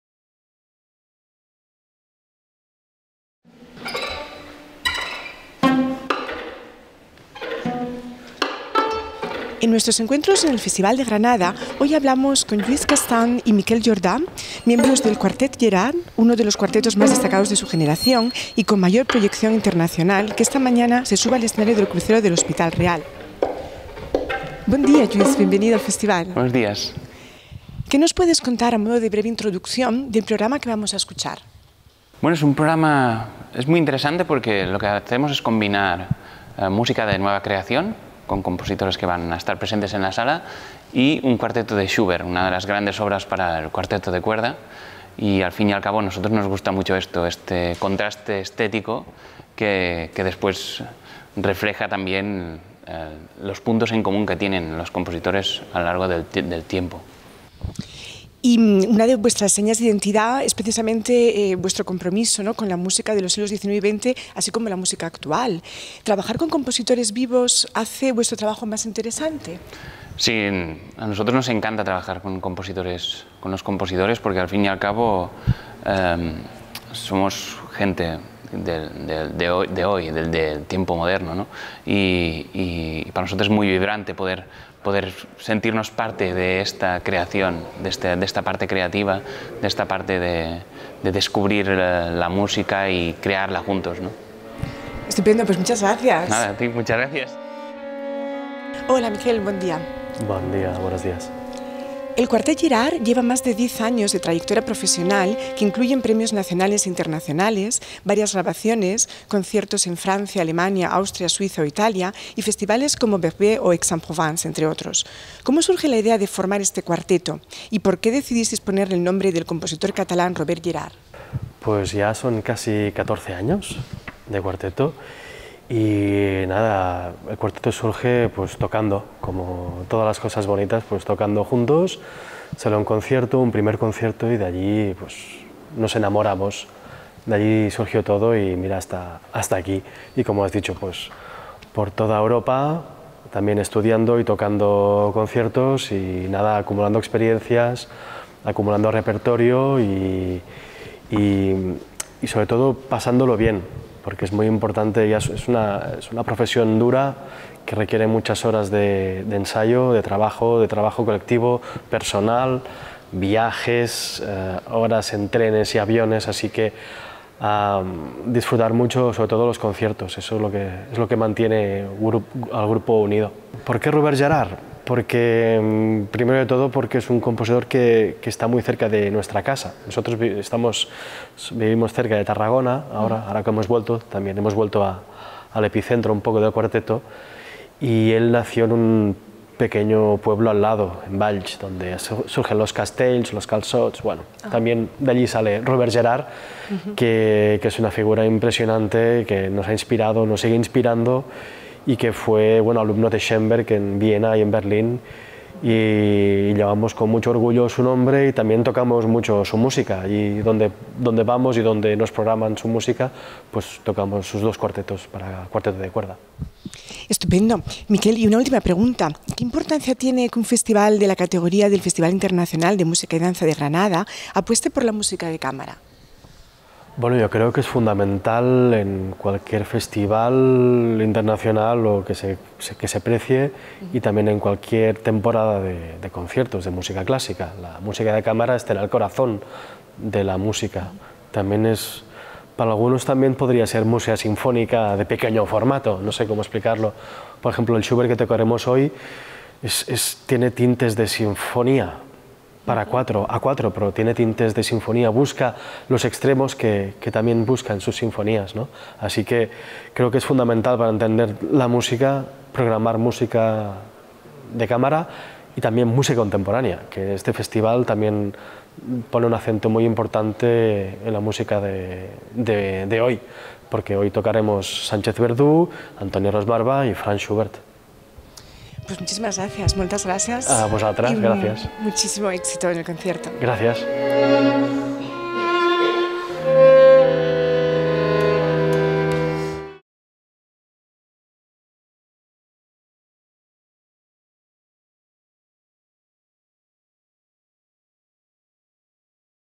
Encuentros en el Festival de Granada - Quartet Gerhard | 73 Edición
Lluís Castán y Miquel Jordà, miembros del Quartet Gerhard, nos reciben en el Hospital Real para hablar del programa del concierto (con obras de José María Sánchez-Verdú, Antoni Ros-Marbà y Franz Schubert), y la interpretación de la música de nueva creación.

Entrevista realizada por Lorena Jiménez, musicóloga y periodista musical

¡Síguenos y ven al Festival! 🤭